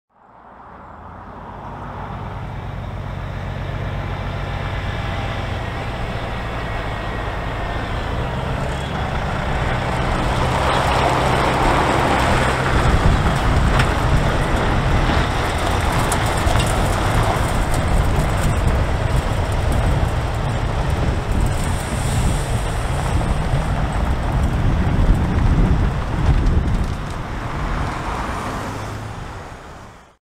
Immaculate Kenworth sprint car transporter enters Australian bullring
On the Australian speedway race transporter spectrum, this sprint car rig is pretty flash. It's a Kenworth T909 prime mover up front, pulling a slick race trailer housing the team's sprint car weaponry. We captured it arriving at Avalon Raceway ahead of the 'Return from Lockdown Cup' in early 2021.